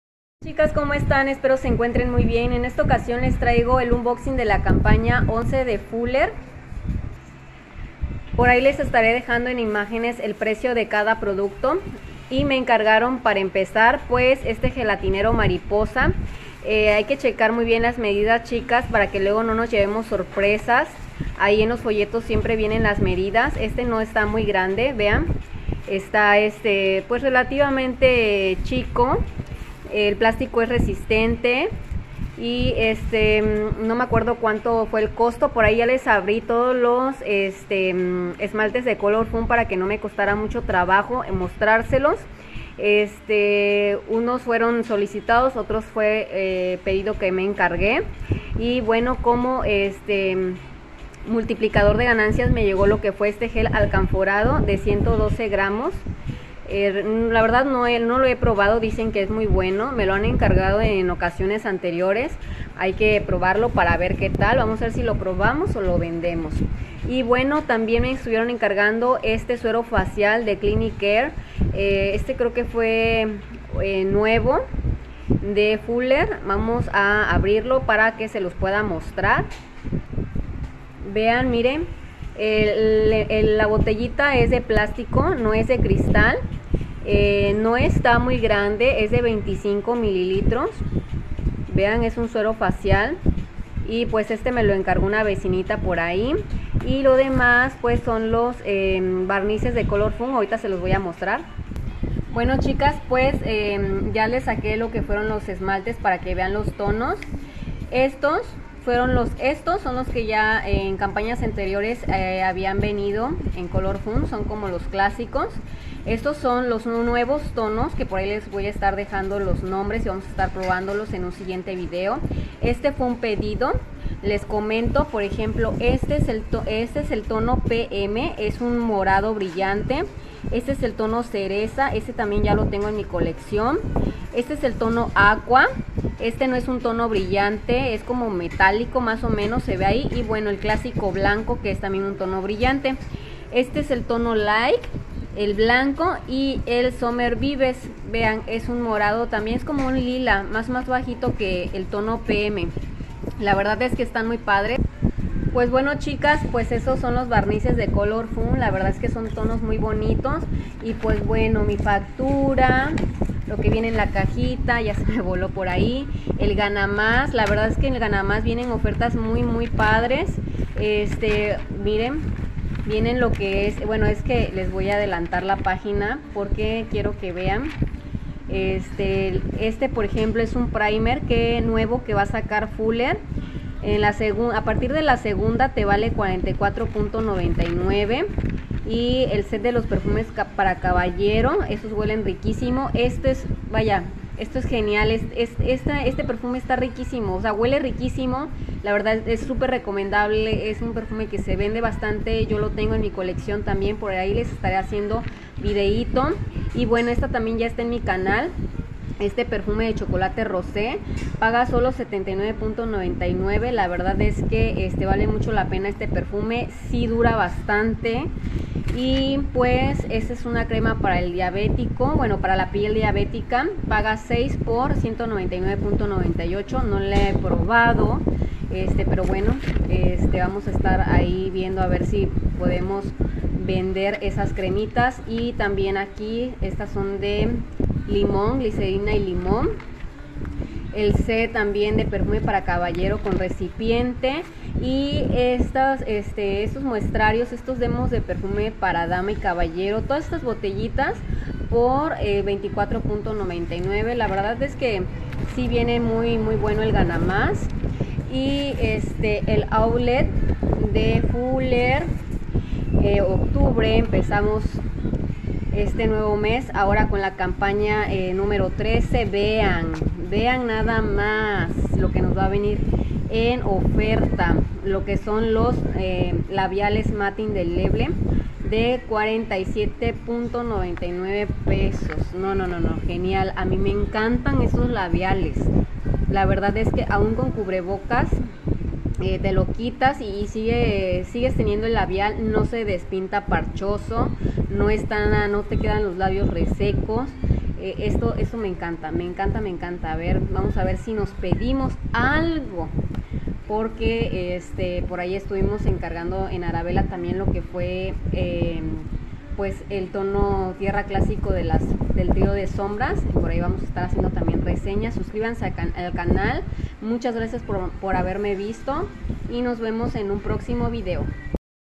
Campaña 11 Fuller unboxing 📦
Productos mencionados
Gelatinero mariposa
Gel alcanforado
Suero facial clínic care
Esmaltes colorfun
Tonos
Cereza
Blanco
Aqua
Light
Vibes
Dark cherry
Deep nude
Grape
PM